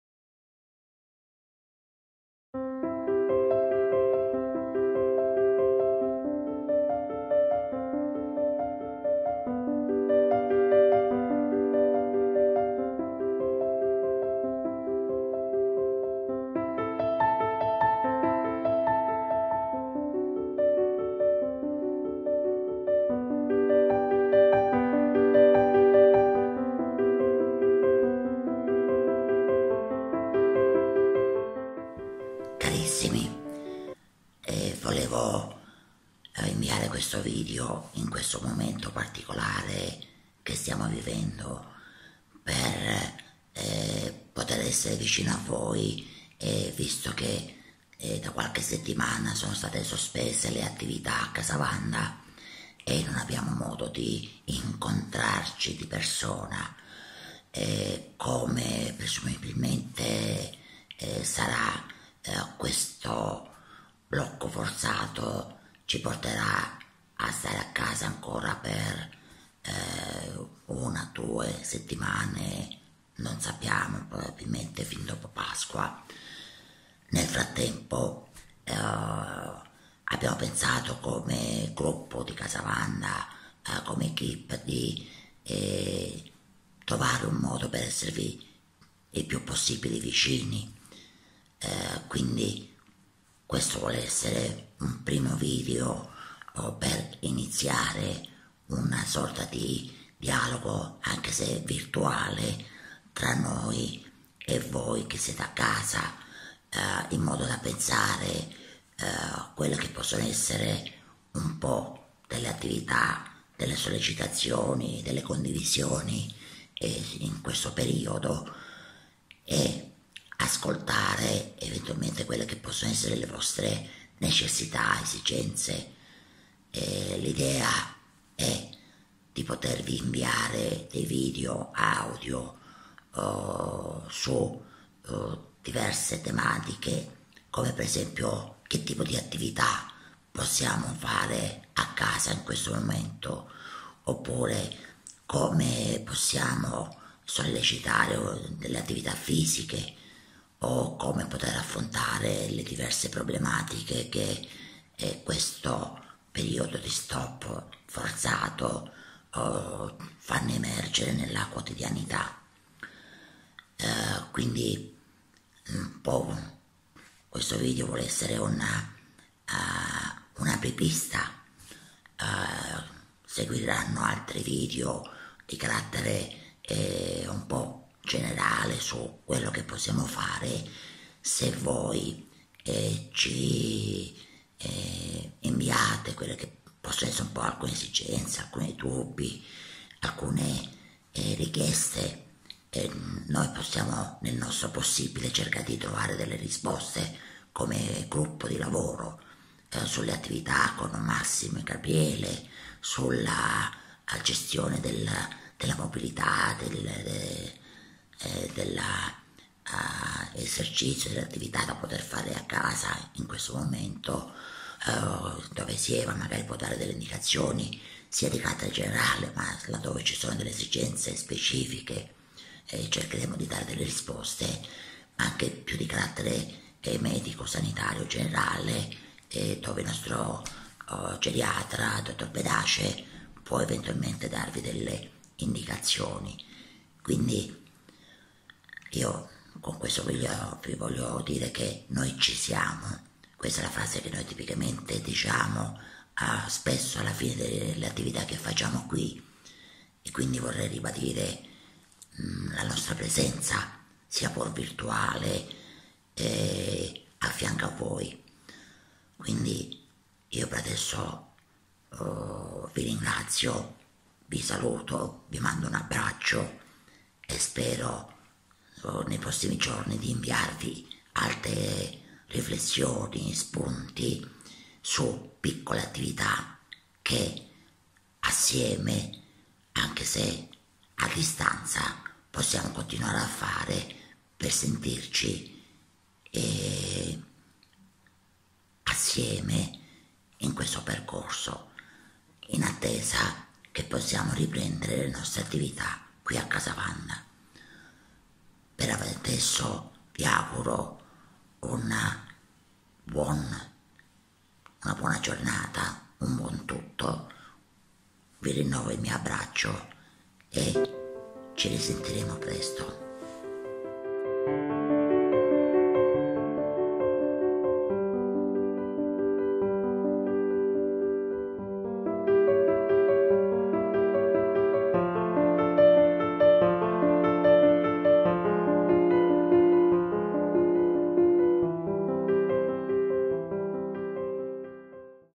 Pillole da Casa Wanda 1 - Presentazione
Casa Wanda è il centro di sollievo per malati di Alzheimer della Caritas di Roma. Aderendo alle indicazioni per ridurre la diffusione del contagio da Covid-19 e a tutela delle persone che frequentano il servizio, dal 9 marzo ha sospeso le attività del centro diurno e dei laboratori. Il periodo di quarantena espone i malati di demenza e Alzheimer, e soprattutto i loro familiari, ad una situazione di stress spesso difficile da gestire. Inoltre spezzare la quotidianità e le abitudini acquisite riduce la possibilità di ricevere stimolazioni esterne al nucleo familiari utili a contrastare il deterioramento delle capacità cognitive e funzionali, ma soprattutto, riduce i momenti di socializzazione.
Per questo abbiamo pensato di creare dei piccoli video, "Pillole da Casa Wanda" da condividere quotidianamente tramite chats costituite su WhatsApp. Un modo per continuare ad essere vicini, dare stimoli attraverso piccole attività e consigli da svolgere a casa.